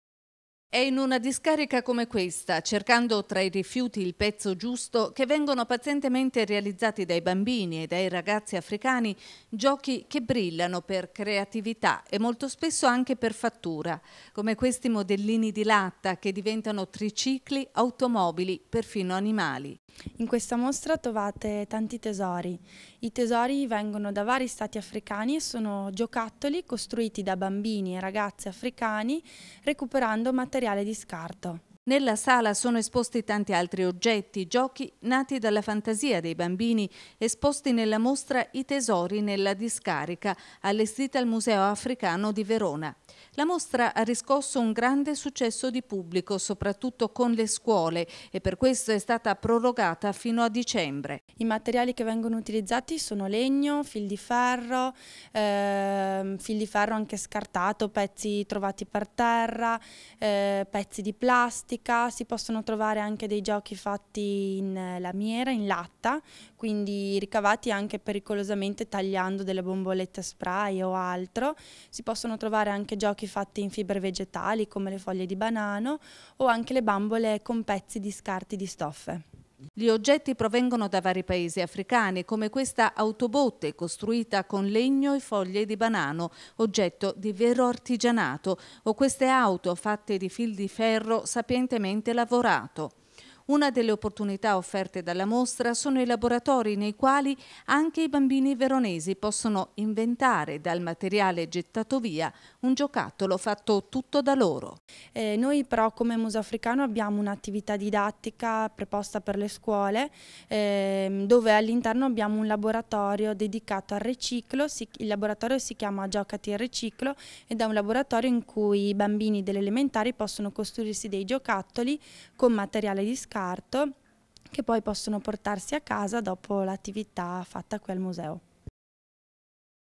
Dai rifiuti alla creatività
" I tesori nella discarica"
mostra al Museo Africano di Verona